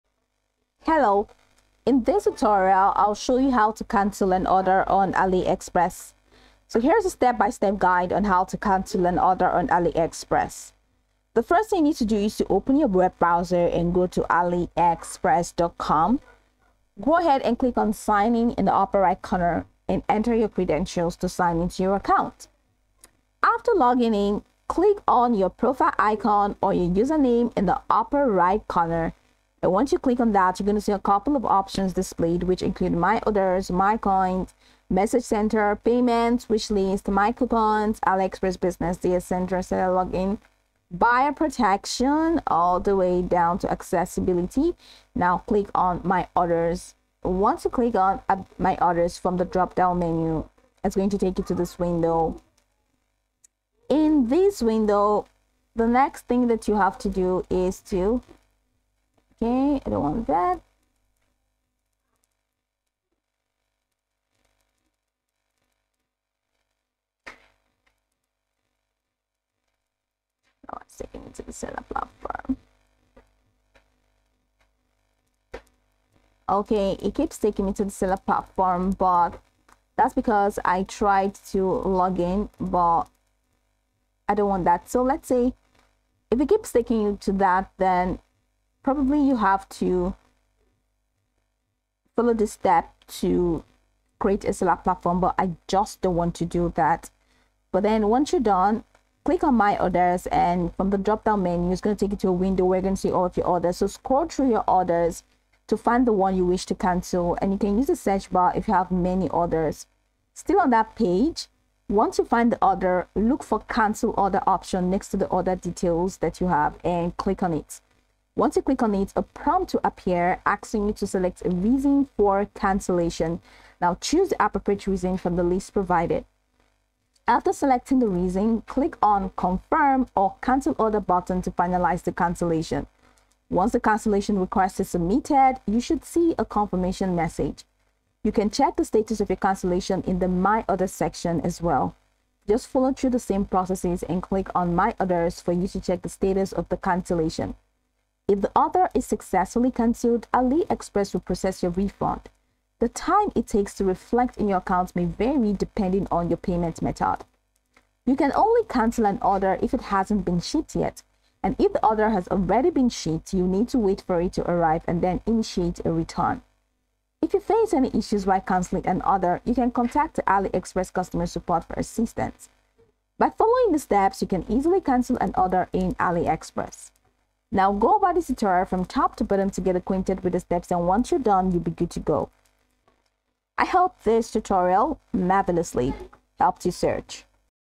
How To Cancel an Order on AliExpress (Easiest Way)​​​​​​​
In this video I will show you How To Cancel an Order on AliExpress. It would be good if you watch the video until the end so that you don't miss important steps.